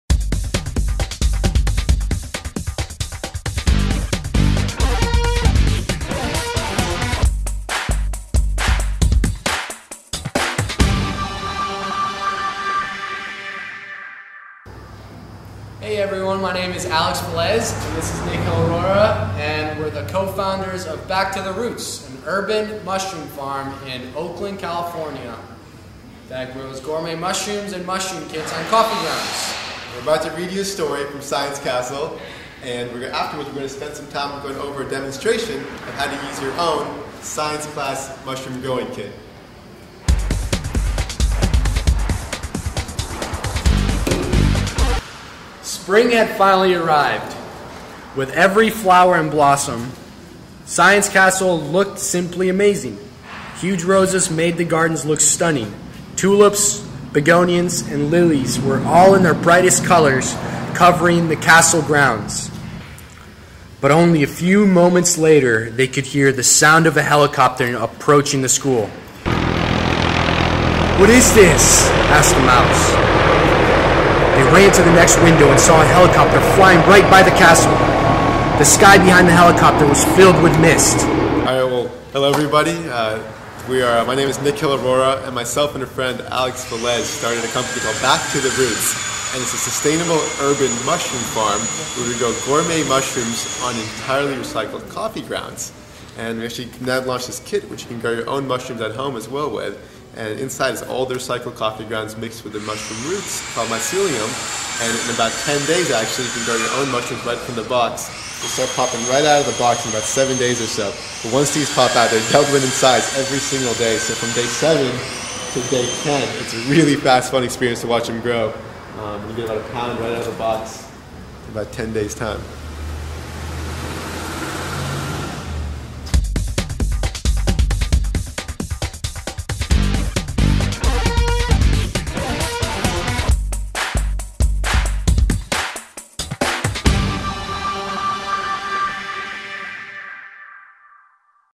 Mushroom Science Kit
- Watch this science class to learn how to grow mushrooms on coffee grounds.